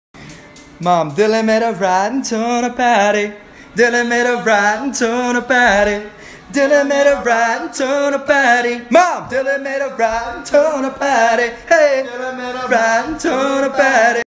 Dylan made a Rotten Tuna Patty
Dylan whipped up a nice expired sea creature. mmm delish.